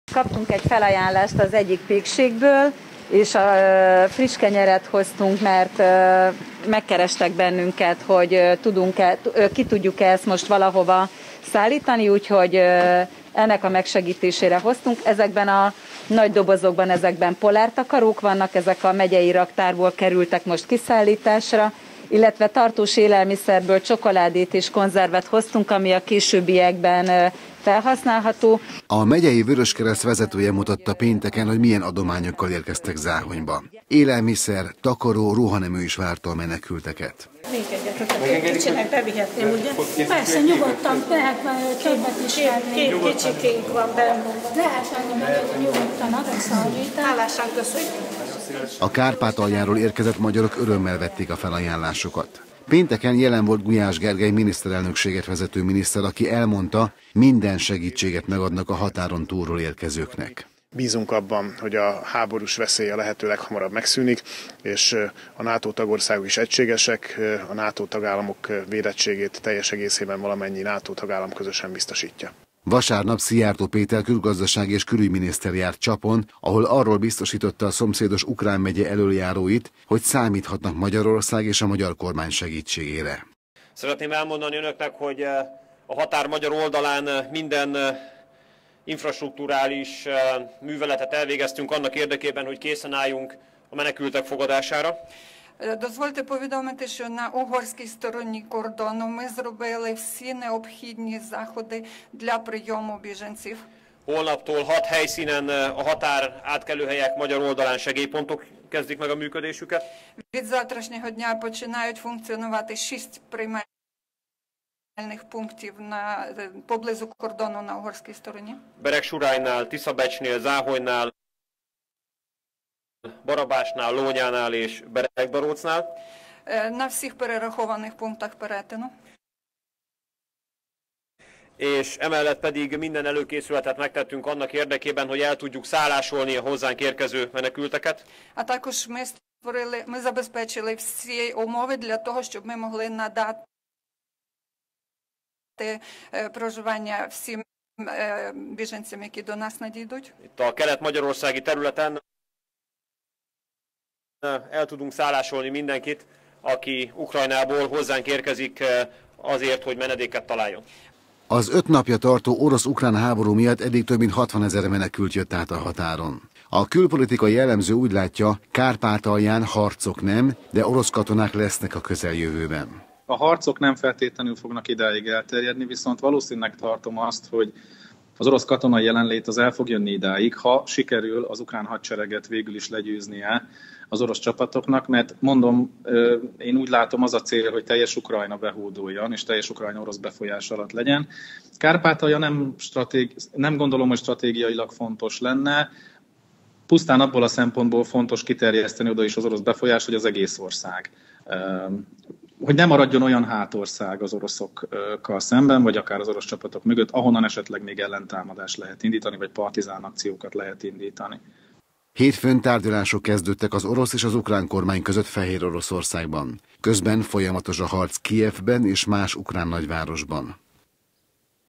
Érkeznek a menekültek
Folyamatosan érkeznek a menekültek csütörtök óta Kárpátaljáról Magyarországra. Szabolcs-Szatmár-Bereg megye mind az öt határállomása éjjel-nappal nyitva tart. Több, mint 60 ezren menekültek eddig. Záhonyban, Beregsurányban iskolákban, művelődési házakban szállásolták el a bajba jutott embereket.

Segélyszállítmányokat is küldenek megyei szervezetek Kárpátaljára. Élelmiszereket, ruházati cikkeket visznek át a határon. Akik segíteni akarnak, hívhatják a 1357-es segélyvonalat. A külpolitikai elemző úgy látja: harcok nem lesznek Kárpátalján, de az orosz haderő megjelenhet a határhoz közeli területeken.